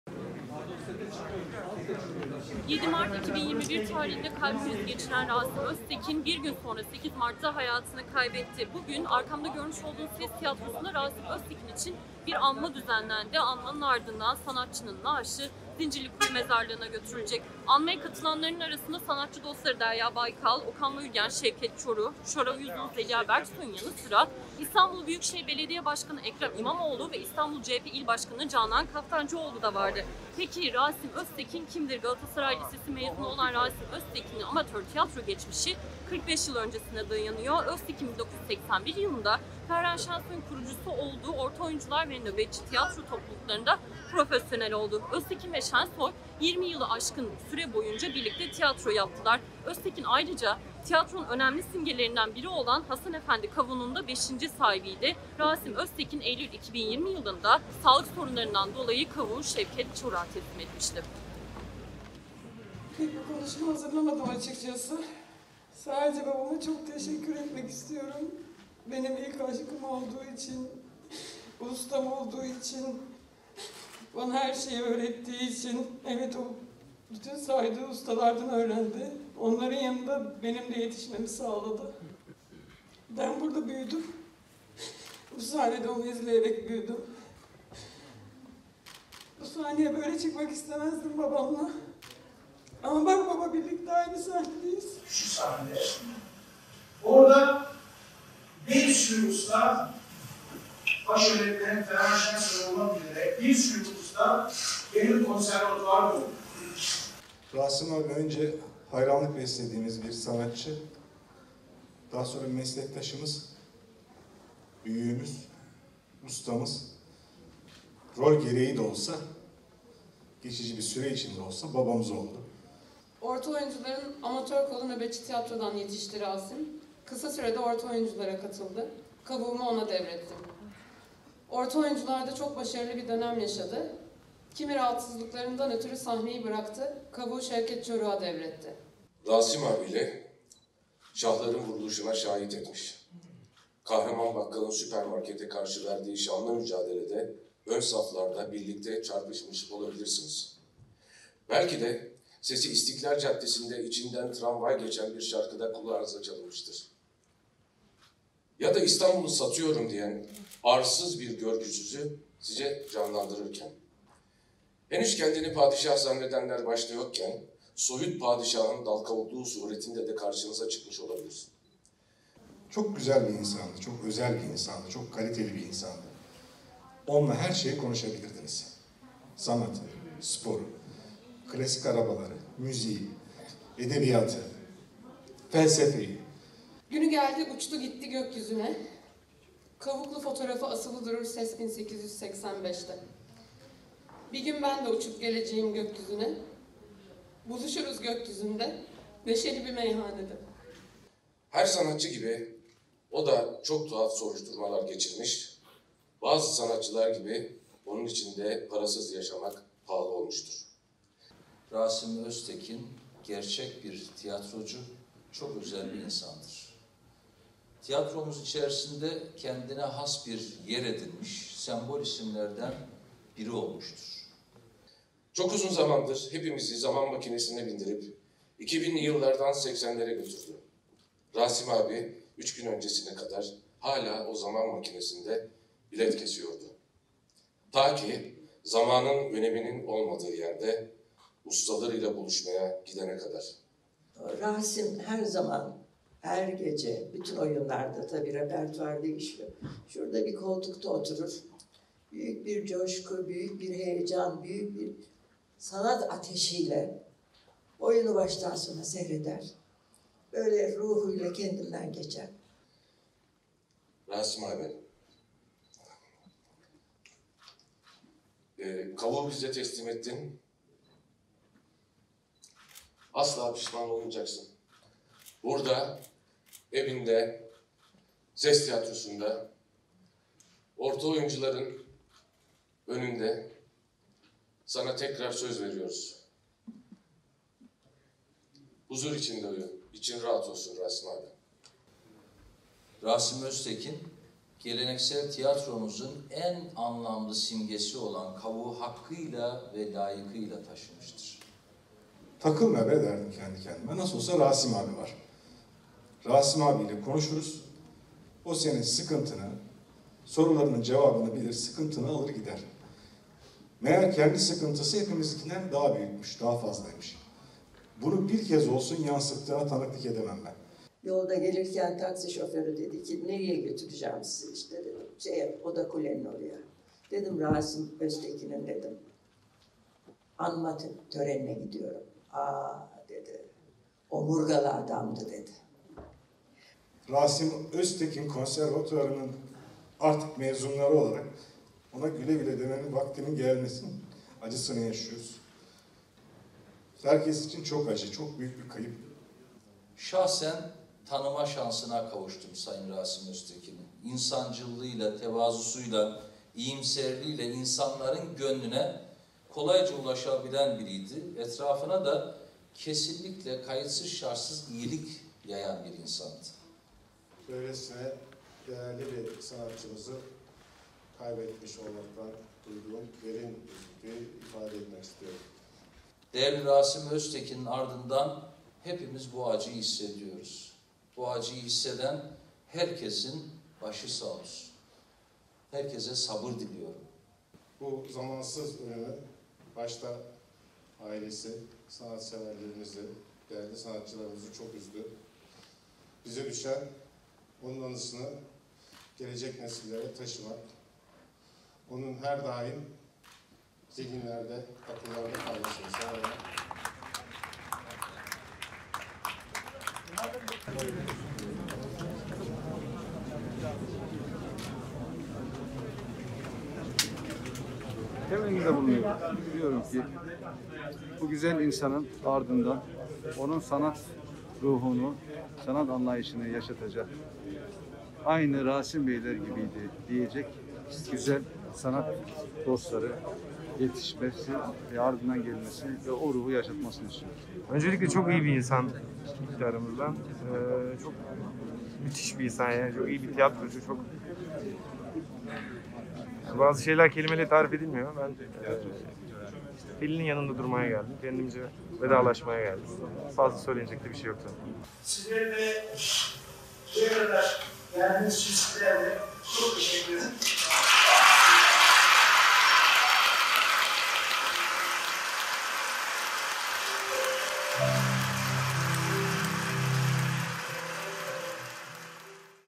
Sanatçı Rasim Öztekin son yolculuğuna uğurlandı
Haber: Sedat Elbasan ve Sahra Atila

Geçirdiği kalp krizi nedeniyle 8 Mart 2021 tarihinde 62 yaşında hayatını kaybeden sanatçı Rasim Öztekin, bugün son yolculuğuna uğurlandı. Ses Tiyatrosu’nda yapılan anma töreninin ardından sanatçının naaşı İstanbul Zincirlikuyu Mezarlığı’na götürüldü. Anmaya ve cenazeye katılanlar arasında Öztekin’in sanatçı dostları Derya Baykal, Şevket Çoruh, Şoray Uzun, Okan Bayülgen’in yanı sıra İstanbul Büyükşehir Belediye Başkanı Ekrem İmamoğlu, Cumhuriyet Halk Partisi (CHP) İstanbul İl Başkanı Canan Kaftancıoğlu, Kültür ve Turizm Bakanı Mehmet Nuri Ersoy ve İstanbul Valisi Ali Yerlikaya vardı.

Teşekkürler!